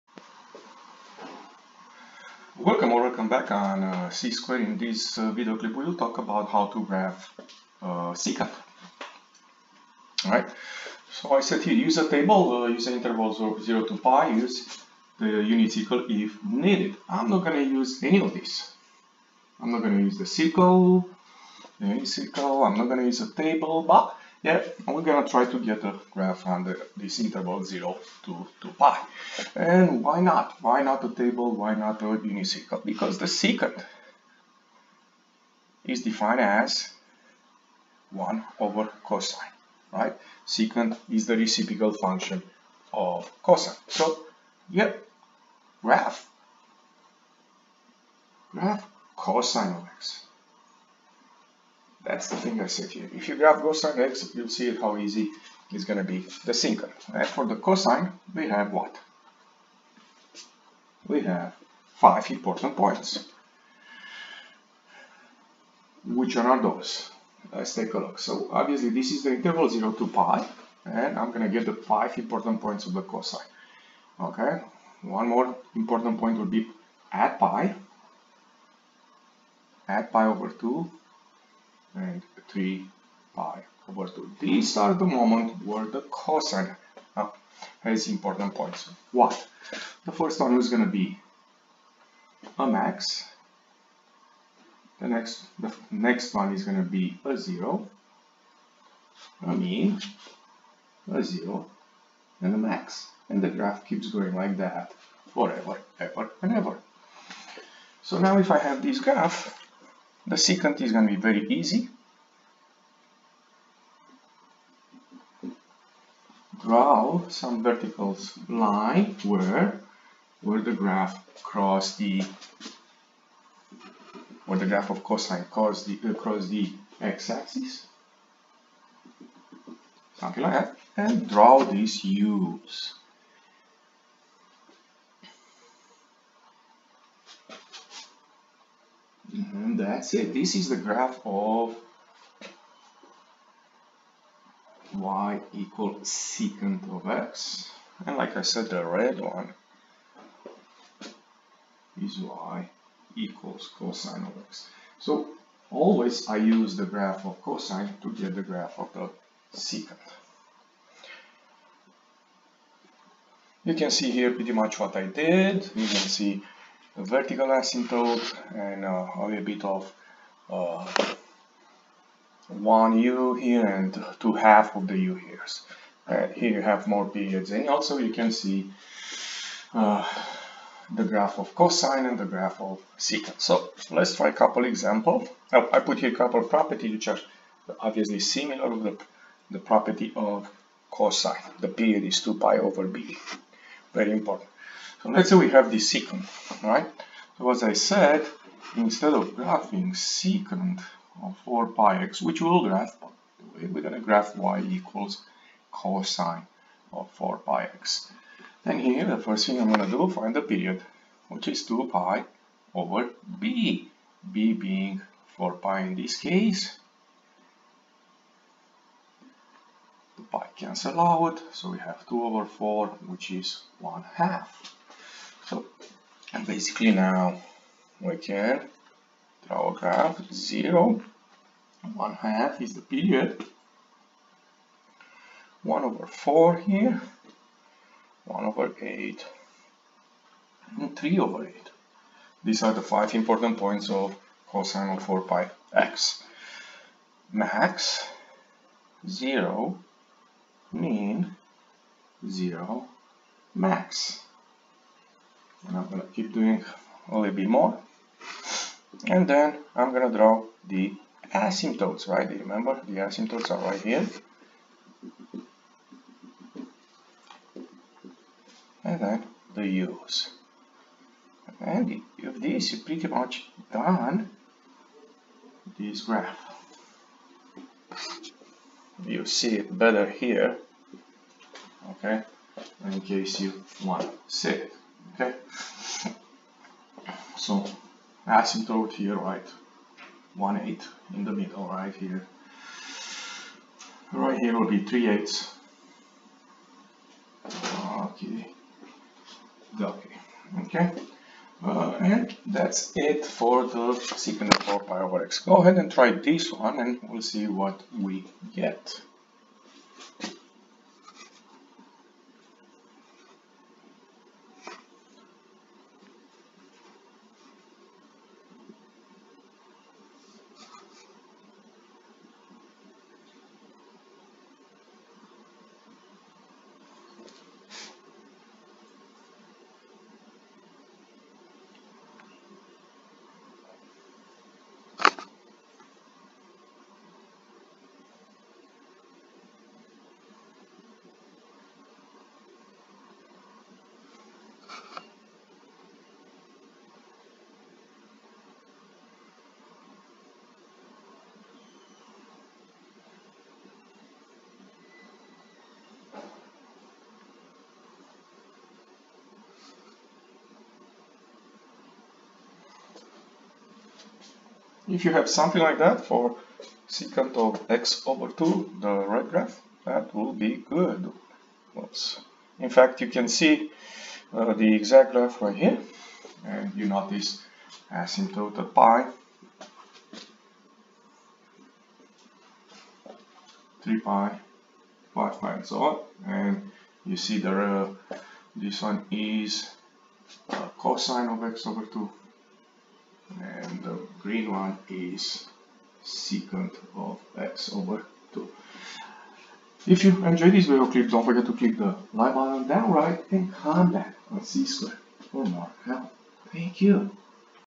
Graphing Trigonometric Functions (Secant)
This clip covers the graph of y=secx using the function y=cosx.